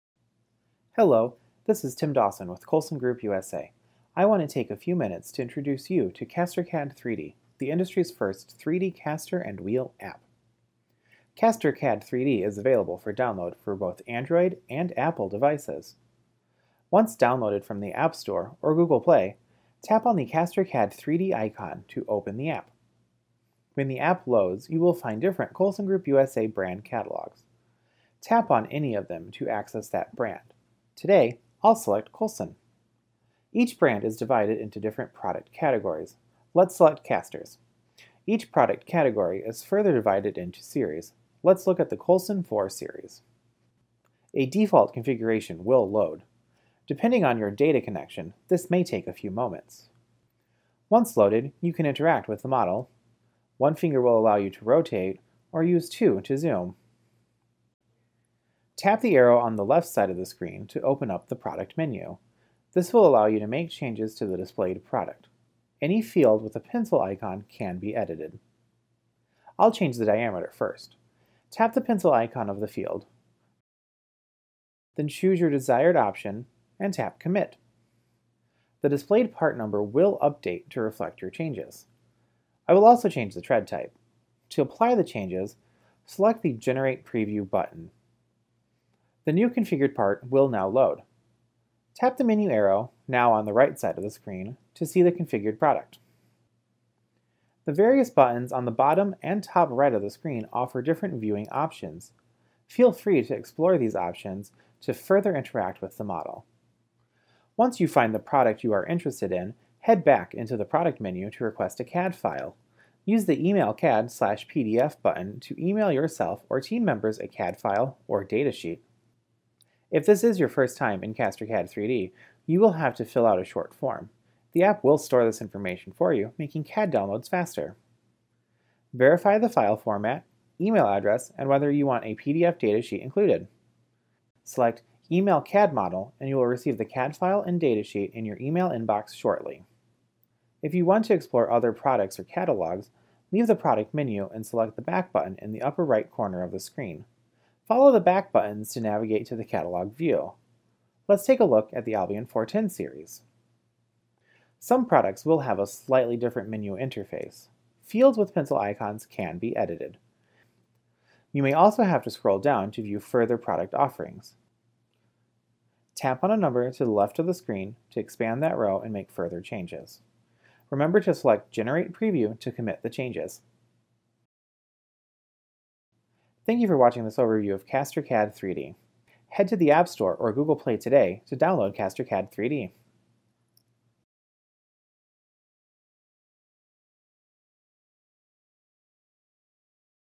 Caster CAD 3D Tutorial
Watch this video for a quick overview of Colson Group USA's Caster CAD 3D App. Learn how to: browse the caster catalogs, configure and interact with 3D caster models, and download native 3D file formats and product datasheets.

Caster CAD 3D is available for free for mobile devices on the App Store and Google Play.

for more information about Caster CAD 3D today.